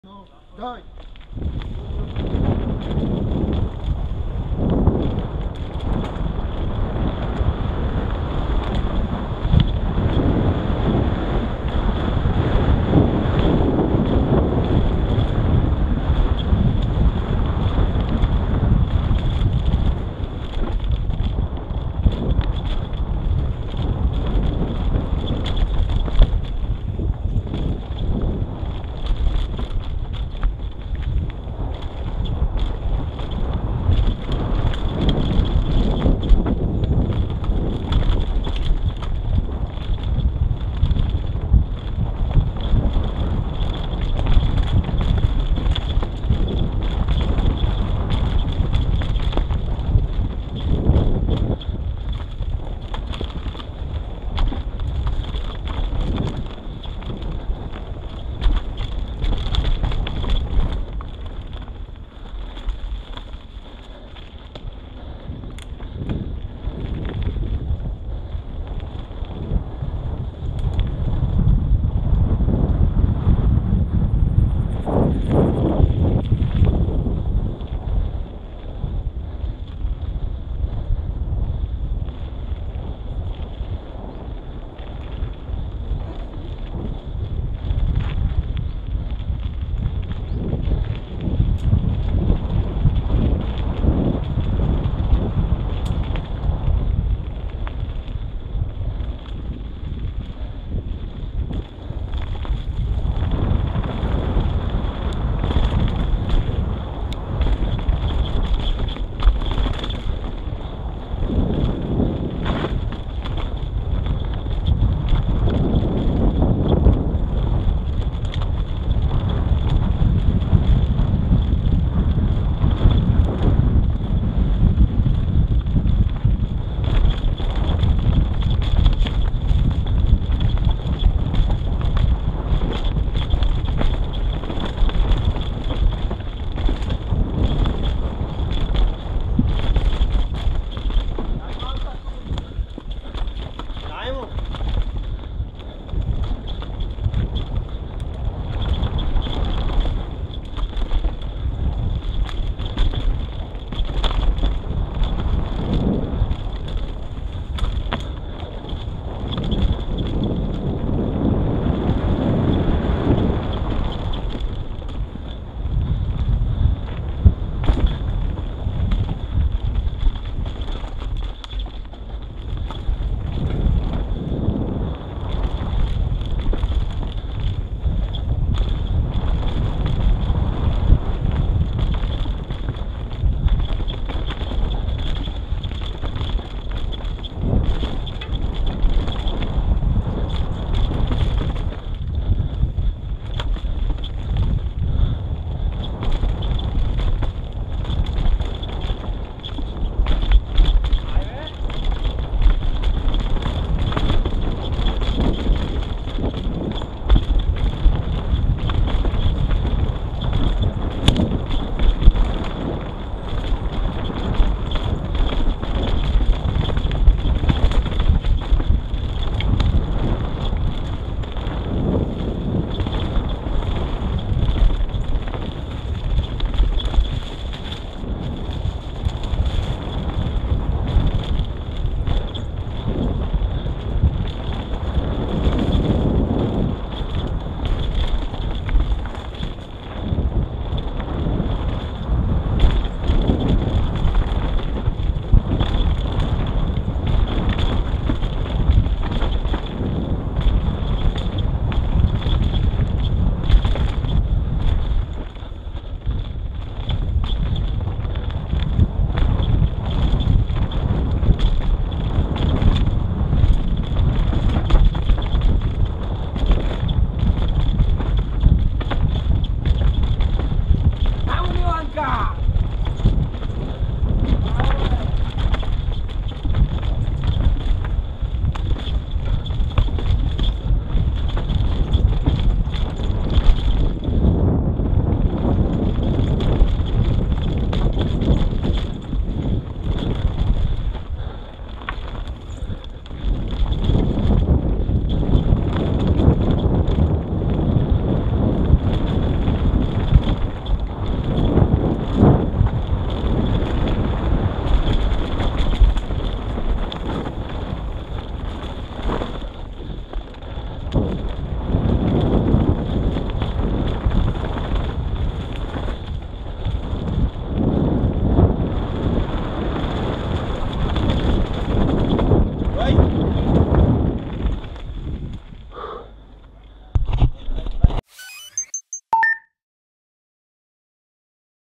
Home Mountain Enduro 2018
crazy race weekend in home mountain enduro :p
i can`t finish all special stages because i broke my front hub in this run. next time more... :D 10x to Garmin